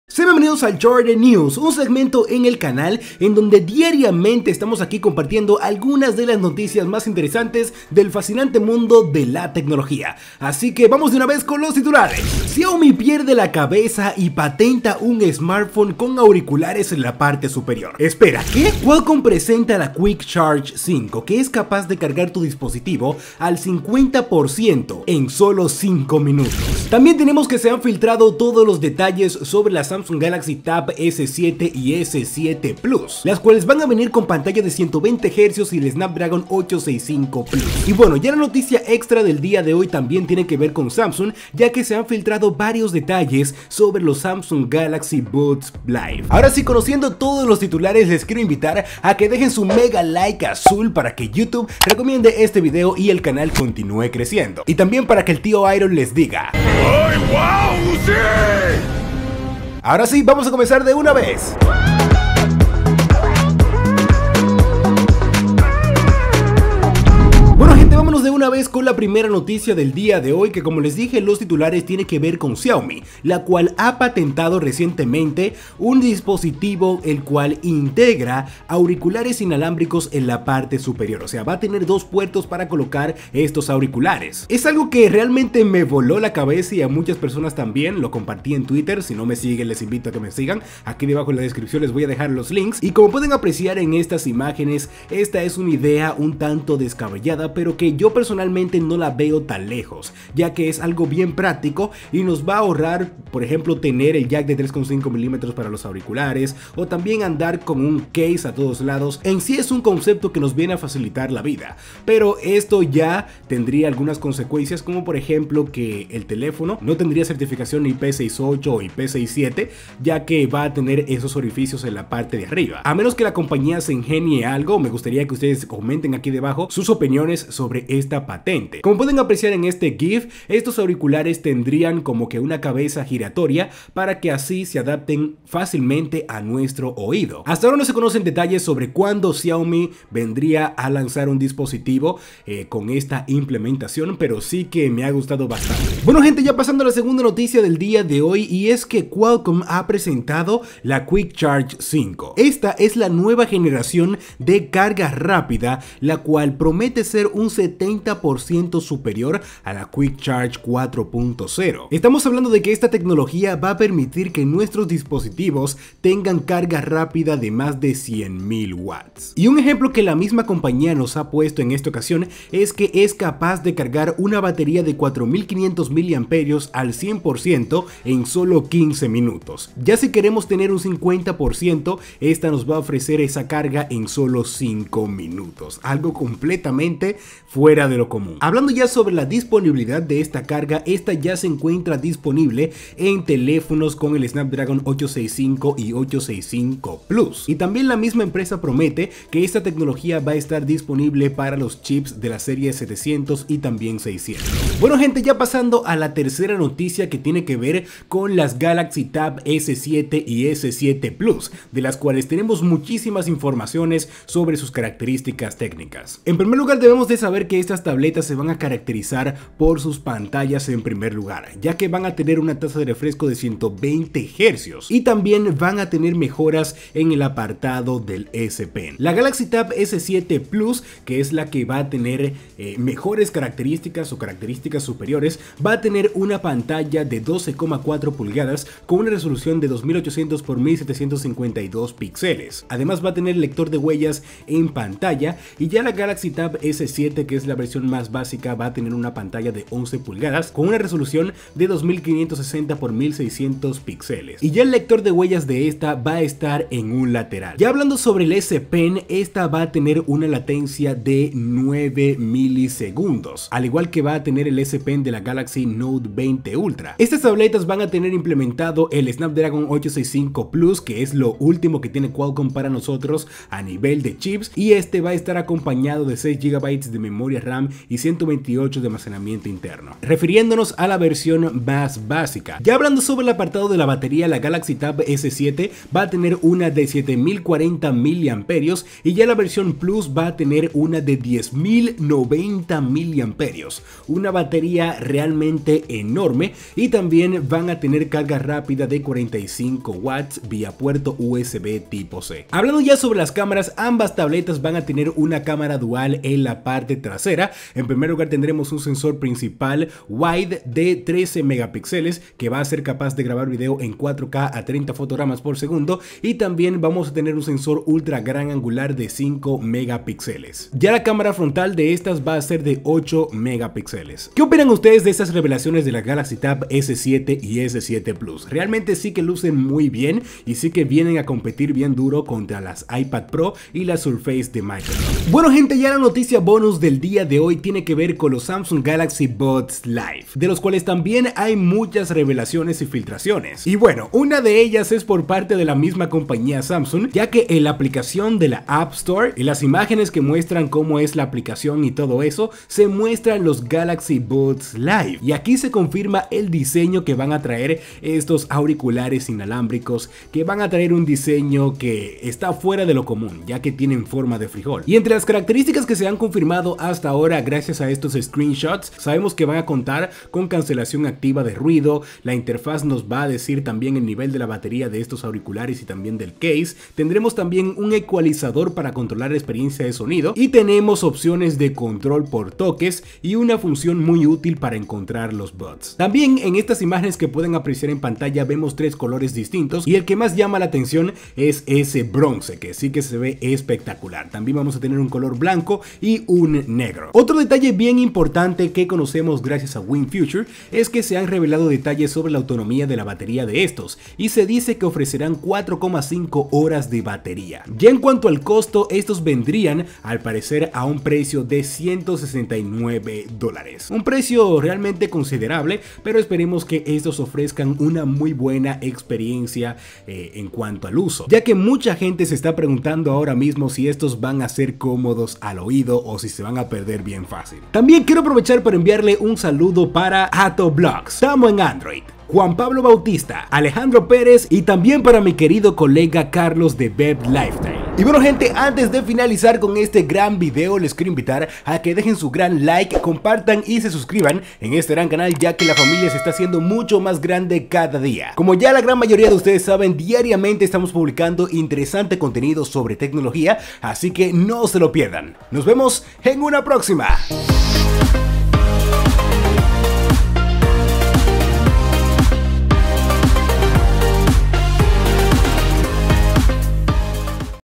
¡Esta Patente de Xiaomi TE VOLARA LA CABEZA! | Auriculares en la parte superior del Smartphone
En el resumen de noticias de hoy hablamos sobre:

-La nueva patente de Xiaomi donde tendriamos auriculares inalambricos en la parte superior del smartphone